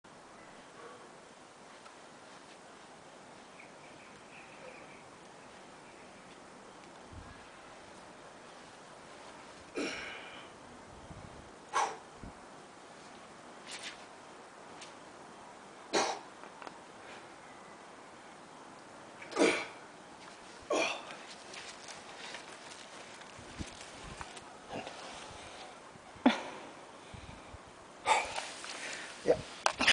Clean and Push Press for the first time with my new 40kg Kettlebell
Today i tried to push press my Bulldog and on the third try i went up then i did it on the left arm to ;)